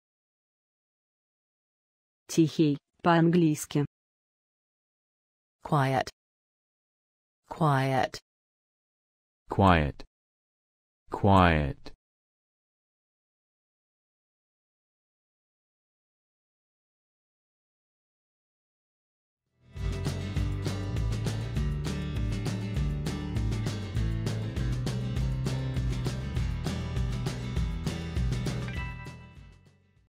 Тихий по-английски
Как правильно сказать Тихий по-английски? Слушай вместе с нами.
А как будет Тихий в переводе с русского на английский? Говори, как произносят американцы. И еще, как правильно написать слово Quiet?

Спасибо!